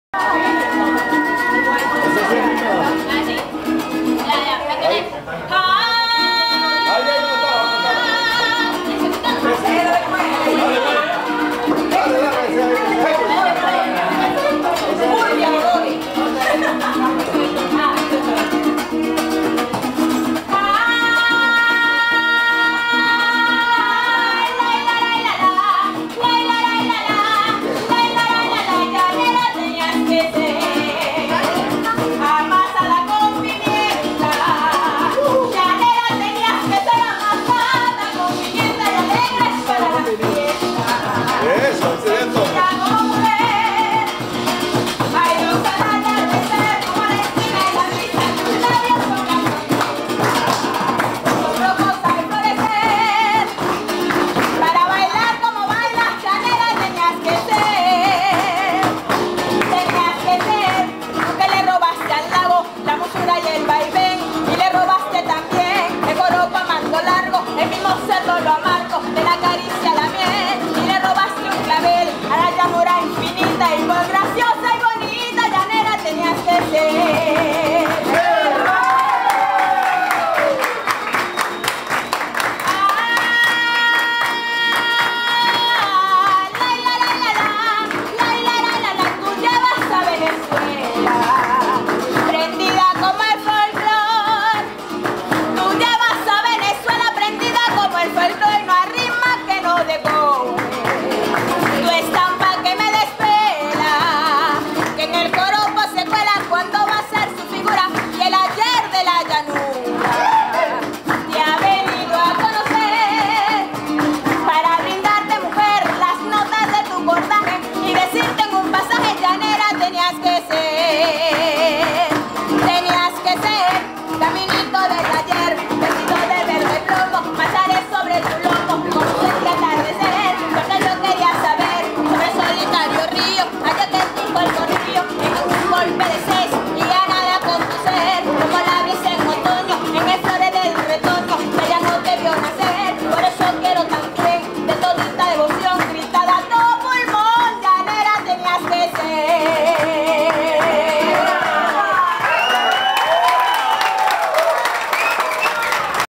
Juliana Paola Martinez canta un corrio
Juliana Paola Martinez canta un corrio. Orfeón Universitario de la UCLA. Aregue 31 Julio 2011